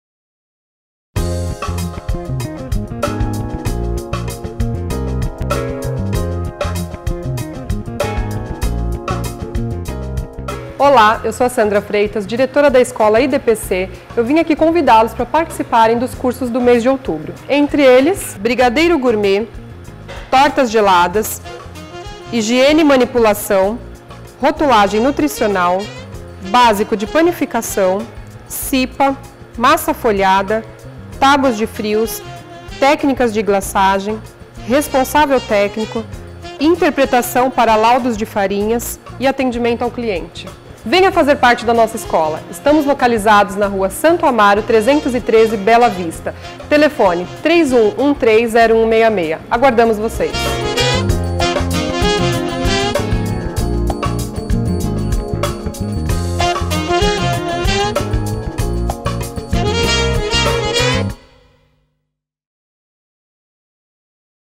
CURSOS ESCOLA IDPC OUTUBRO 2016
Conheçam os cursos que ESCOLA IDPC preparou para ministrar no mês de outubro de 2016 . Faça sua já inscrição .